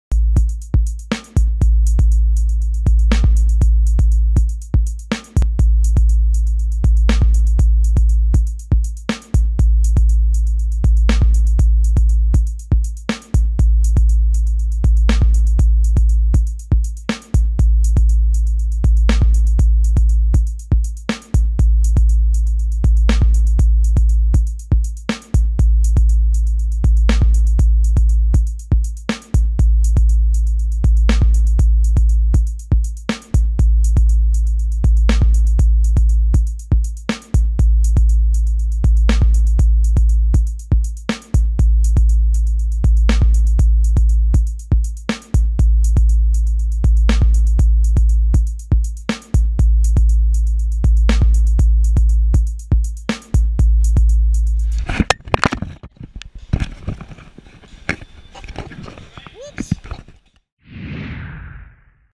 BAM Academy Balancing POV
Dana working on some balancing technique at various heights